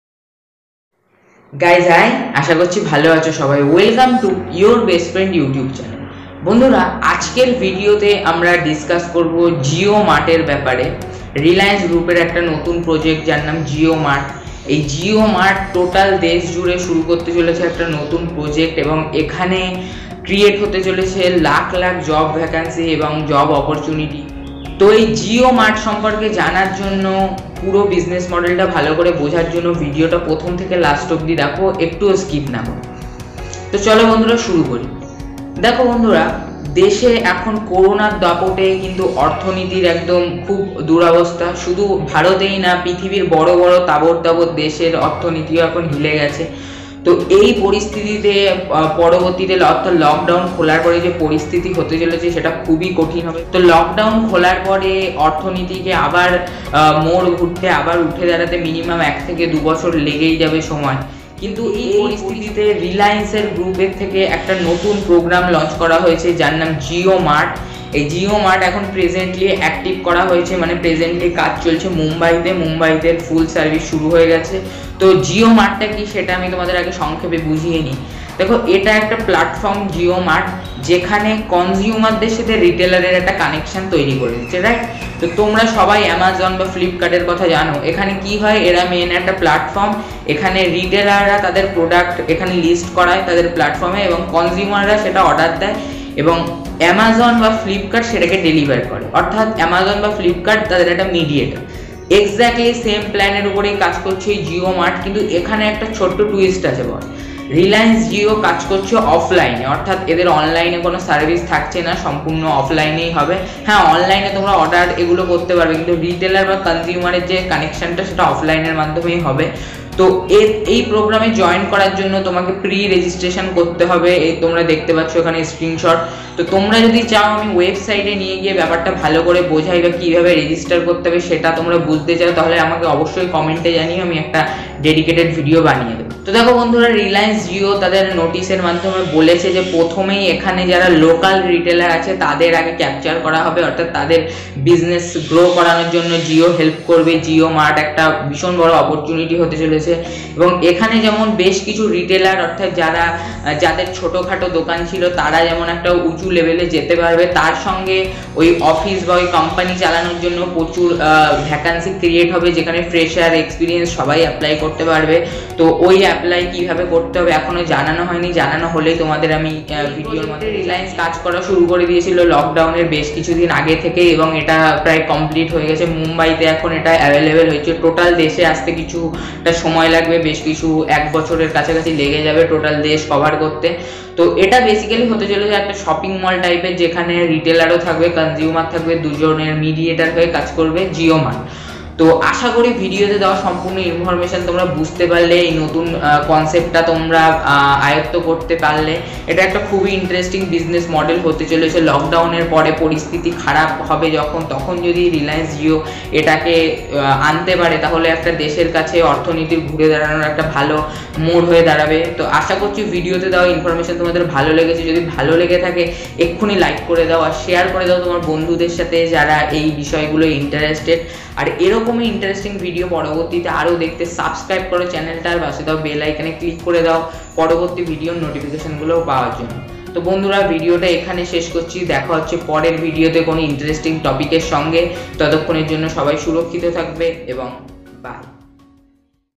Best Job opportunitiy After Lockdown | Work from Home | 10th Pass | Jio Mart
Hi guys 👋.

লকডাউন এরপরে দেশের অবস্থা কিন্তু খুবই খারাপ হতে চলেছে লাখ লাখ মানুষ চাকরি হারাবে এবং সমস্যা সৃষ্টি হবে প্রচুর সেই সমস্যা থেকে বাঁচার জন্য জিও একটা নতুন প্রজেক্ট লঞ্চ করেছে যার নাম জিও মাট, তো এই জিও মাটের ব্যাপারে সম্পূর্ণ তথ্য নেওয়ার জন্য ভিডিওটা মনোযোগ সহকারে দেখো!